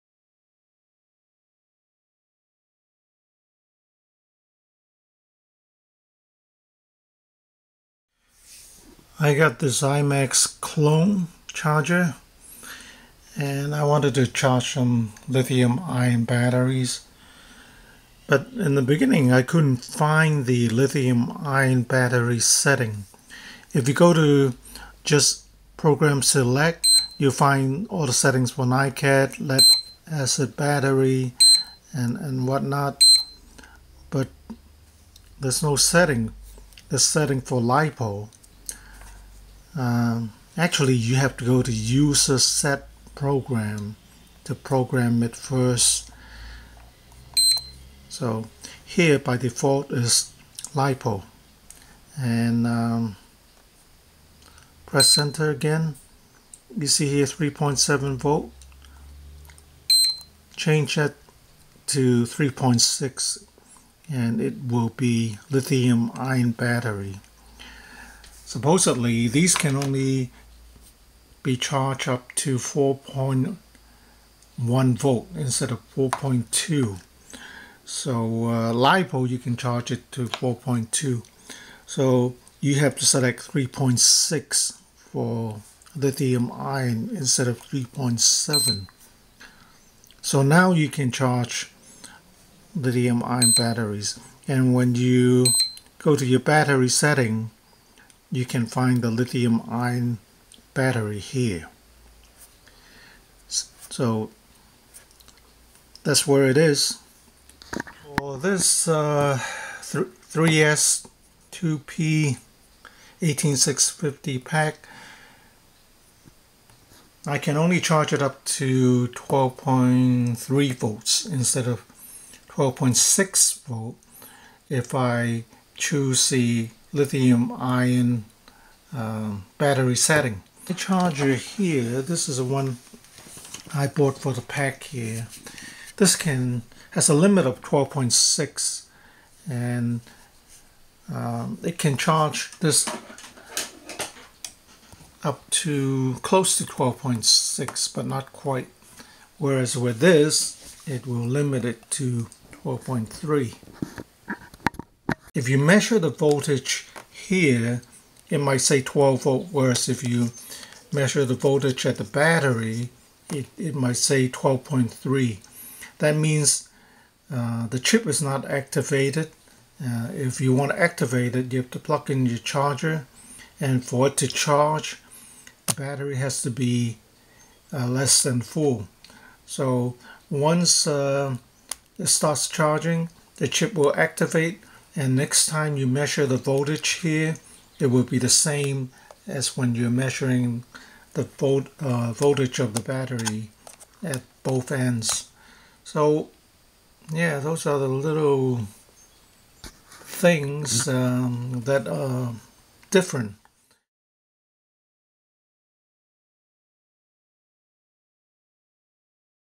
where is the LiIon battery setting in the imax B6AC clone balance charger/discharger
I bought the imax Dual Power B6AC balance charger so I can charge my batteries more precisely.  Since I paid $27 for it, it is probably a clone version.  The discharge function can help to determine the amount of charge held by your cells.
  Since I have some 18650 batteries, I needed to charge them but I couldn't find the right settings for them with this charger.
  Apparently, the settings for LiIon batteries are grouped under LiPo battery setting by default. You can go to USER SET PROGRAM.  The default is LiPo and voltage type is 3.7V.  Press enter and then the increase button and scroll to 3.6V, press enter to save.  When that is selected, the lithium battery setting is set to LiIon instead of LiPo.

 When you go to battery type (red button),  under Program select, you will now see the option for LiIlon battery.  Select that if  you want to use the setting. The lithium ion battery will be charged to 4.1v only and a pack of 3s cells charged to 12.3v.  If you have nominally rated 3.7v 18650s, you can select LiPo setting to charge to 4.2v.